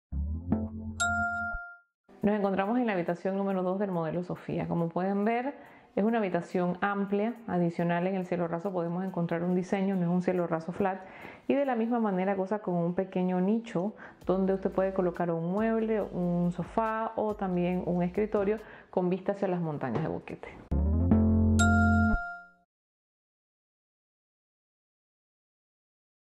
Recámara #2 Boquete Canyon Village Prestige Panama Realty. 6981.5000
La recámara #2 del Modelo Sofía en Boquete Canyon Village es amplia, incluye todos los clostes completos.